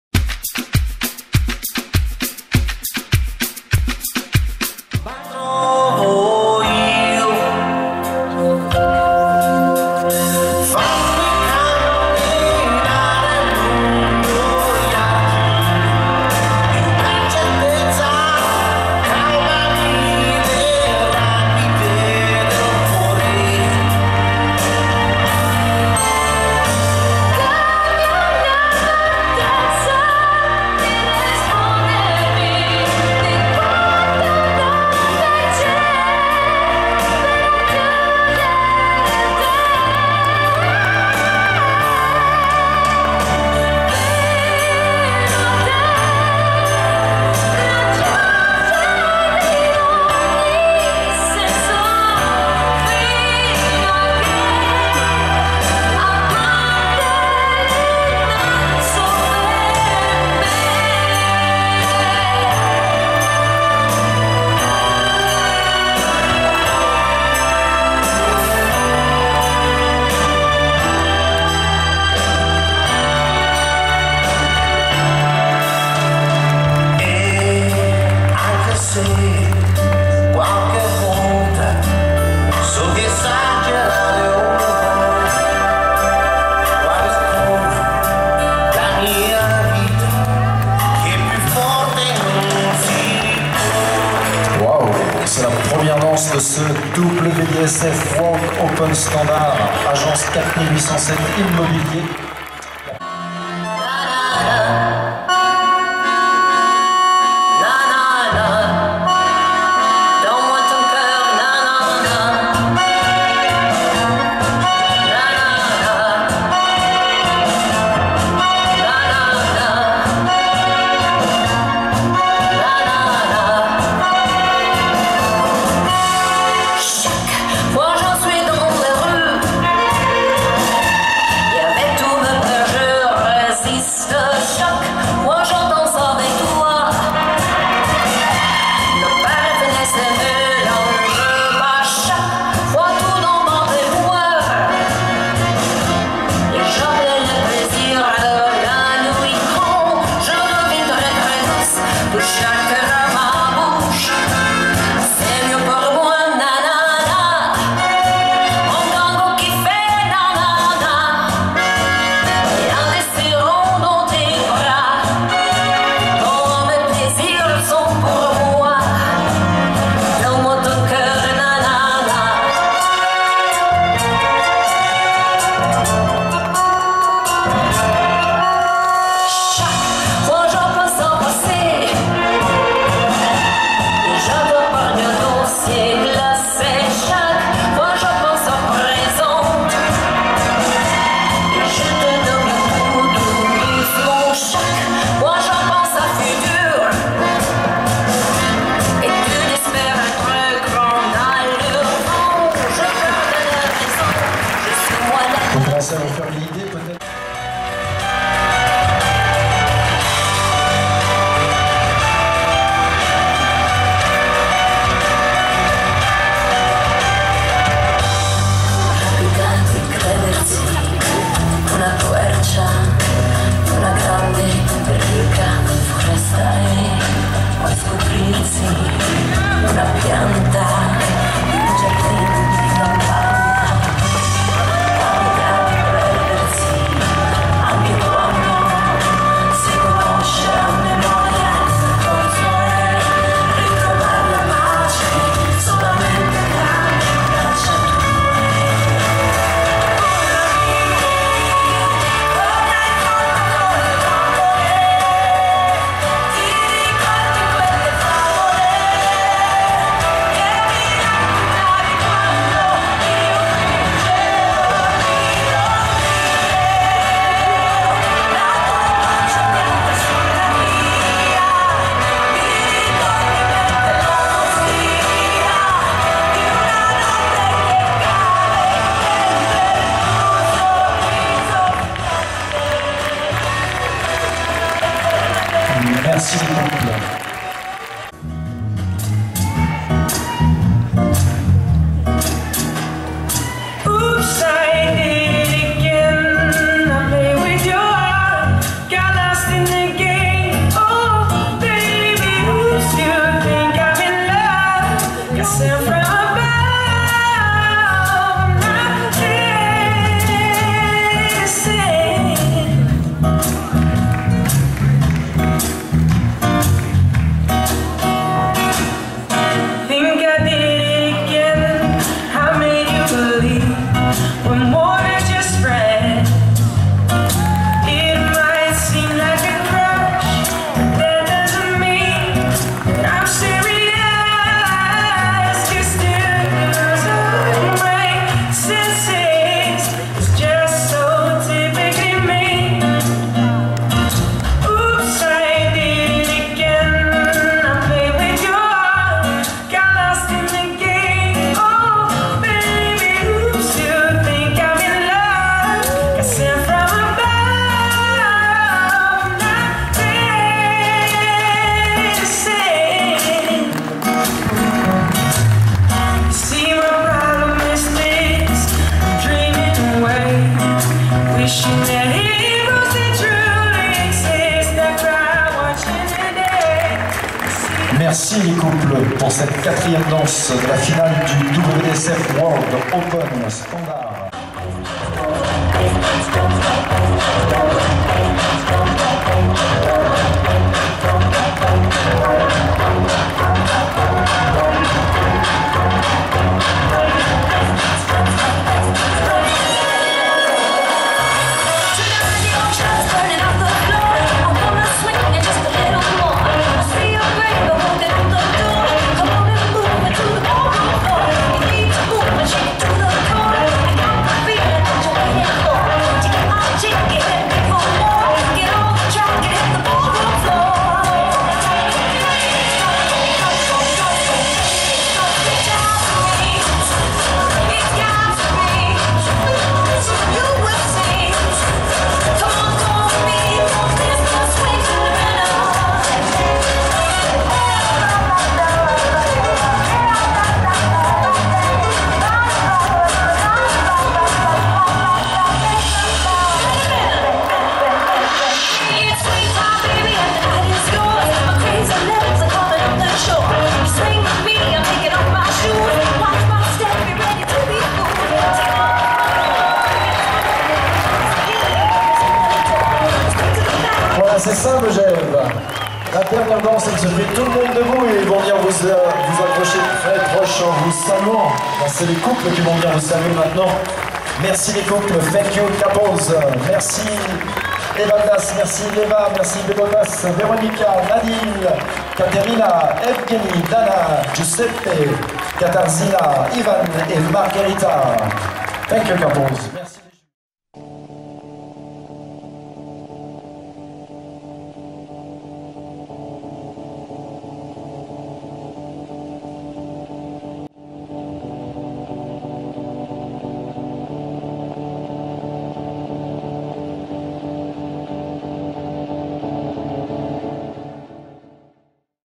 DSFO Megeve 2014 | WO STD | Final
Final of the WDSF World Open Standard at the Dance Sport French Open 2014 in Megeve/France, from 11th to 13th of April.